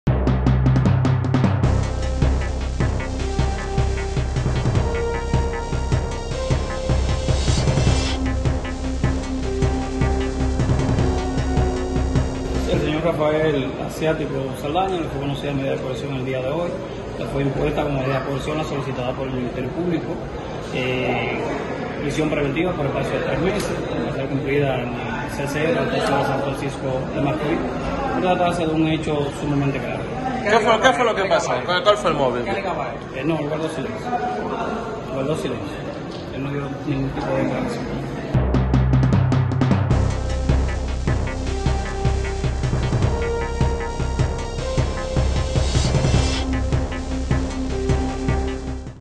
Prisión preventiva a hombre que hirió su esposa en la cabeza en SFM
San Francisco de Macorís.- Fue impuesta la prisión preventiva como medida de coerción, contra un hombre quien el pasado 20 de abril hirió a su esposa de un balazo en la cabeza en el paraje Las Caobas.

Se trata de Rafael Asiático Saldaña, quien cumplirá la medida de coerción por espacio de tres meses en el Centro de Corrección y Rehabilitación Vista al valle, por el hecho que cometió dentro de su hogar contra Yolanda Aracena Grullón.

La medida de dictada por la Oficina Judicial de Servicios de Atención Permanente de la provincia Duarte.

A su salida del tribunal, el imputado se limitó a ofrecer declaraciones sobre el caso que aún no ha sido esclarecido por las autoridades.